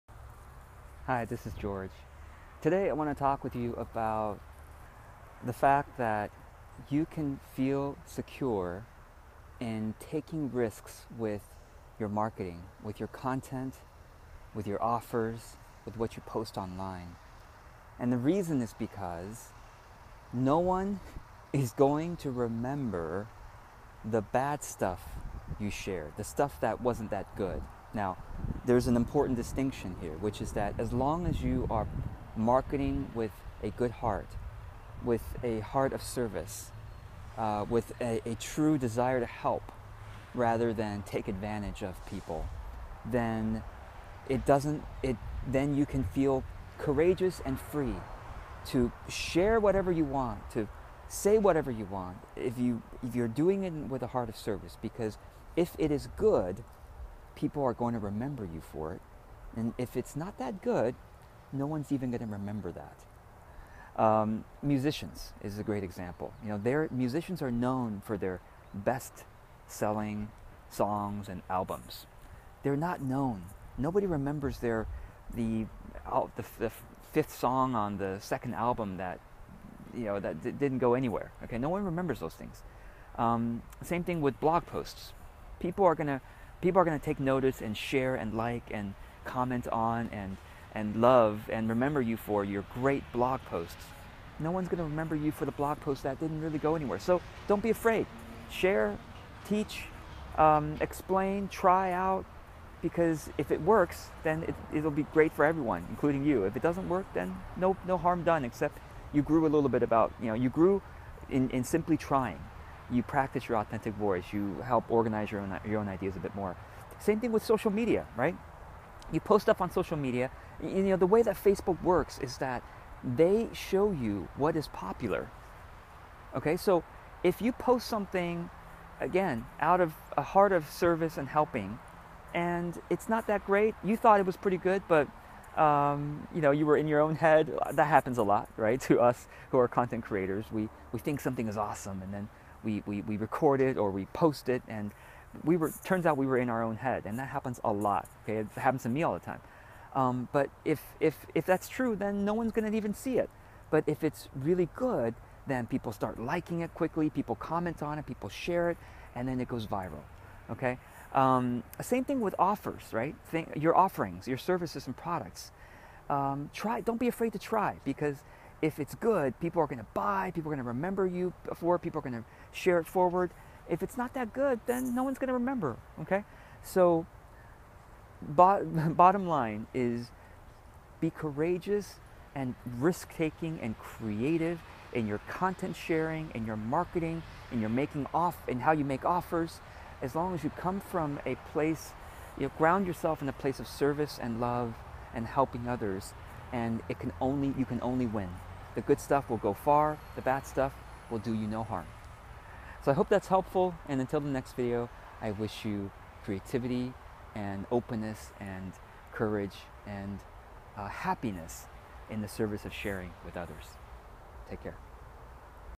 No one will remember any of your content -- except the good stuff -- so share from the heart!
When it comes to content creation, here is a harsh truth... but at the same time, truly wonderful:

No one is going to remember *any* of your content, except the few pieces that are *really* good.

For example: music artists.  Their popular songs are what they're known for.  Their unpopular songs no one remembers.

It's true on social media too.  Facebook features the popular stuff.  Barely anyone will see the posts that got few or no likes.

Blog posts, same thing.  If people love it, and they link to it, Google notices and bumps it up in the search rankings.

*What this means is that you can feel liberated to share* whatever you believe to be helpful, important, interesting, funny... anything you believe is worth sharing.

*...because it's not up to you to decide.*

Your role is to create and share.

Your audience's role is to decide whether that content is worth remembering!

If it is worth it, they will definitely let you know by their likes, comments, and shares.

If however you don't see much engagement, it's the audience's gentle feedback that they didn't think much of it, no matter how important it felt to you.  So what you do is simply *move on* to creating or sharing the next thing.

Trust me, there have been many things I've shared that I felt were really important... but you didn't think so, and many others in my audience thought nothing of those pieces.

It's not your fault, or anyone else's.

It's simply the constant illusion that we content creators experience: the ugly baby syndrome.

Just because I created a piece of content, it feels important to me.

Same with you: if you spent time recording a video or writing a blog post, it naturally feels more important to you, than to anyone else who consumes it.

All we can do is to share content with our audience and let *them* tell us (through their likes) whether it's objectively worthwhile.

*If we hear silence* it means that we were in our own heads when we made that content.

...or, that we didn't share it with the right audience.  The content may have been good, but it wasn't relevant for *this* audience that we shared it with.

Sometimes, we get *lucky* and our content is loved by our audience.

(We can dramatically increase our luck by sharing lots of content!)

When that happens, our content will be picked up by the internet and social media algorithms, and generously shown to others who *weren't* part of our audience... new people who could decide to join our tribe!

However, most of the time (this is simply statistics) our content *doesn't* go viral.  It might be barely liked by our audience.

But here is the silver lining:

Nobody will remember those "meh" pieces of content.

So be open and liberated to share as much content as you want!

...taking into account not to inundate your audience with too much frequency, however.

There is an important underlying point here:

As long as you ground yourself and your actions *in a spirit of service and helping, then you can only win.*

You either learn... or you get good results.

Whenever you share content, you are learning, simply because you are practicing.

If your audience *happens* to like it, you win because more people are seeing it.

If no one likes it, you *still* win because by simply creating or sharing content, you are getting practice expressing your voice and experimenting how to resonantly share your ideas.

However; if you do not ground yourself in a spirit of true service and helping, but instead you come out of fear or greed, and you take advantage of people, they will remember.

I made this mistake in the first few years of my business.

I shared content and offers primarily out of a deep fear (that I wouldn't make enough money or get enough popularity) or out of greed (want to make even more money or be even more popular)... and really, I ended up taking advantage of people.

Back then, I was freely sharing only teaser-content, not the real stuff.  That good stuff?  I made my audience *pay* for.  I pretended to give "free trainings!" but they were a carefully designed ploy to get them to buy the real content later.

Many of my previous audience members never forgot.

Even though I'm in my 7th year of business, it feels like only 2 years ago, I started over completely in building a new audience, one that I would truly serve and help, who could truly trust me.

Though it was painful, I am glad to see that now.  I am grateful for the spiritual growth I've experienced.

It feels like such a blessing now to create and generously share, out of a spirit of service and helping.  It fills everyone's heart who benefits, including mine.